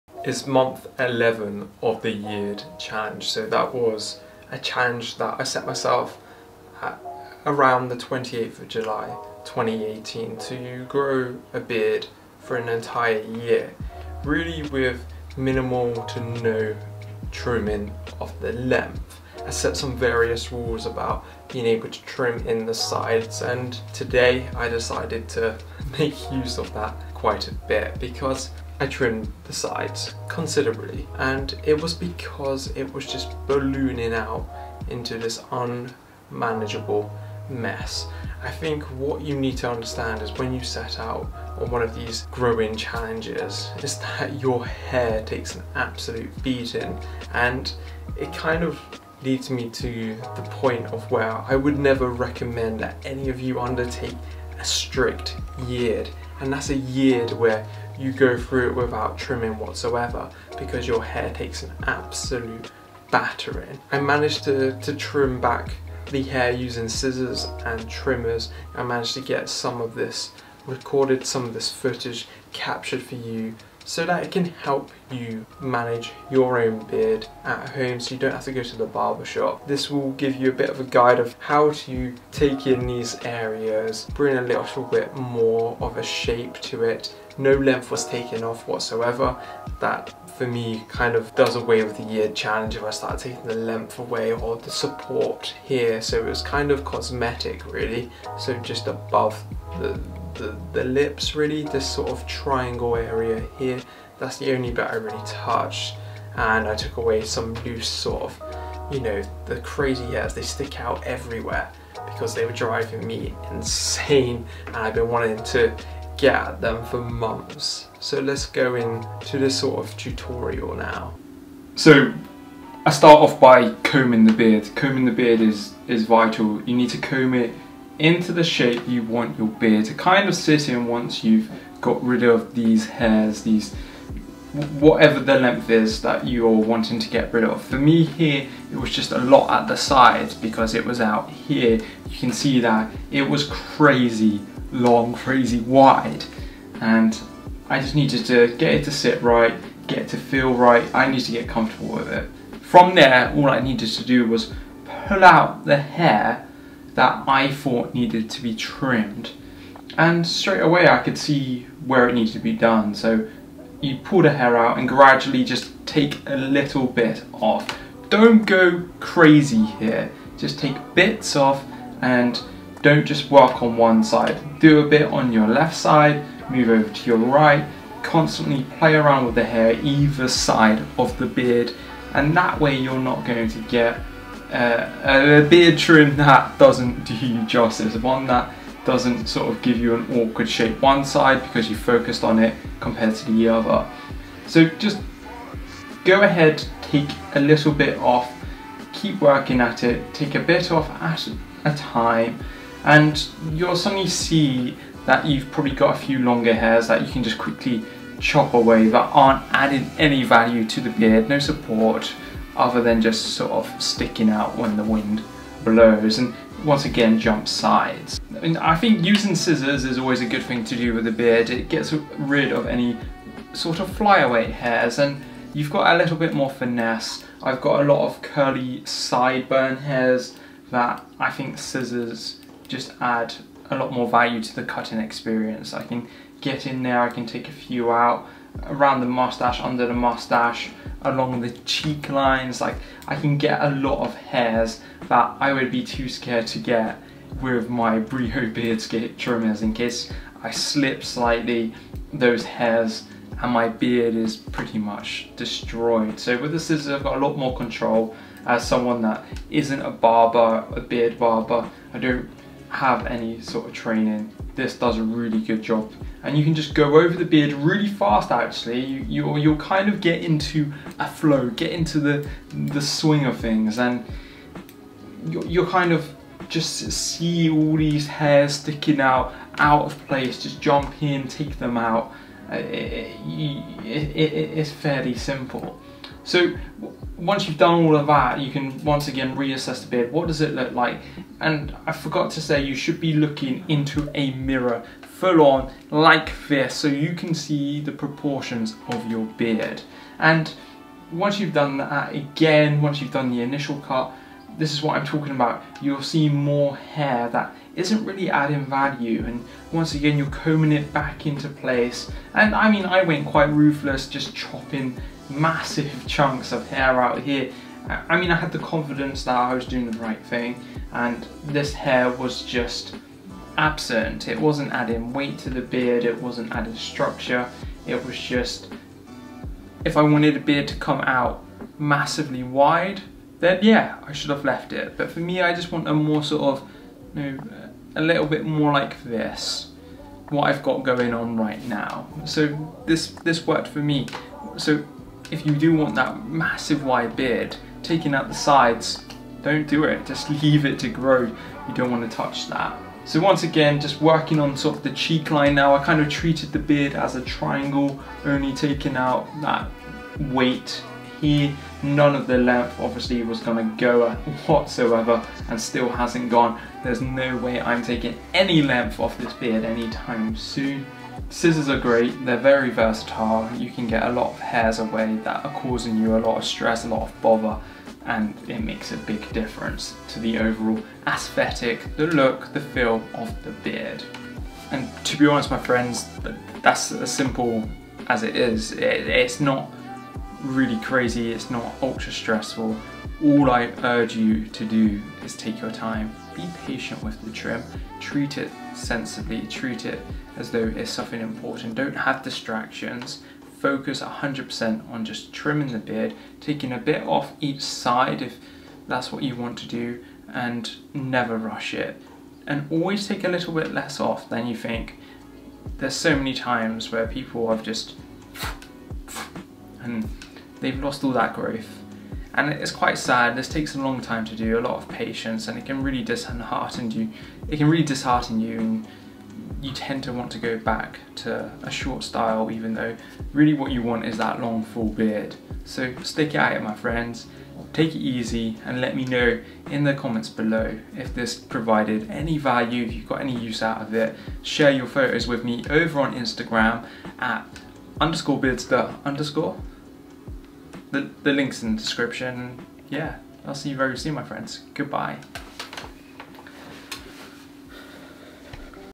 How to Trim a Long Beard at home with Scissors and Clippers - Yeard Month 11
In this tutorial, I teach you how to trim your beard with scissors and clippers. The beard is at 11 months of growth and this is a long and full beard.
Jump to the tutorial at 01:48

Trimming the beard takes a fair amount of practice. But my best advice when trimming your beard at home is to take your time. Never rush a beard trim as it is likely that you will trim far too much beard hair off. And that sucks!

Investing in a good pair of hair or beard scissors is going to give you the tools to get the beard trimmed and looking very good. Make sure the scissors are sharp and capable of cutting beard hair with one snip.

You want to think about the shape you are trying to generate the with the beard trim. That beard needs to look good so try to get the style of the beard right and don't overt rim trying to get the beard style right.